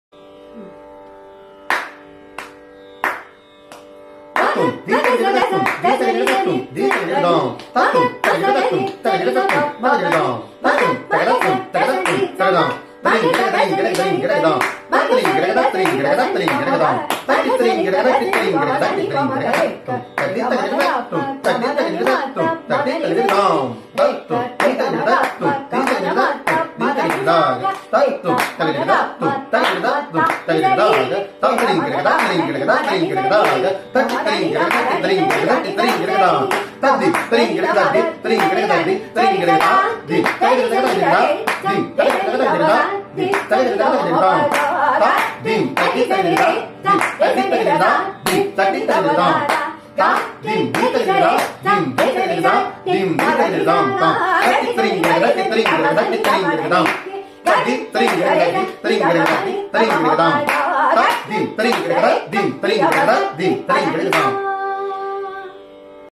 Musicmatics#2 Thirunageswaram TRSubramanian's perspective of 3, 4 and 5 as 3 parts of Korvai !!
3 parts of the korvai - amrutavarshini- hamsanandi - simmendramadhyamam

1- purvangam starts with 3,uttarangam starts with 3, karvai 3 and avartanam 3
2- purvangam starts with 4, uttarangam starts with 4, karvai 4 and avartanam 4
3- purvangam starts with 5, uttarangam starts with 5, karvai 5 and avaratanam 5

- Logically and aesthetically crafted by Kalaimamani Thirunageswaram Thiru.T R Subramanian !

3 avaratanams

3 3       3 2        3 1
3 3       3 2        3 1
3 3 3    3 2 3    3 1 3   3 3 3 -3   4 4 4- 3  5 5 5

4 avartanams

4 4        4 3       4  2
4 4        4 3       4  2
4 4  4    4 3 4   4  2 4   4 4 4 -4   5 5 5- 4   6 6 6

5 avaratanams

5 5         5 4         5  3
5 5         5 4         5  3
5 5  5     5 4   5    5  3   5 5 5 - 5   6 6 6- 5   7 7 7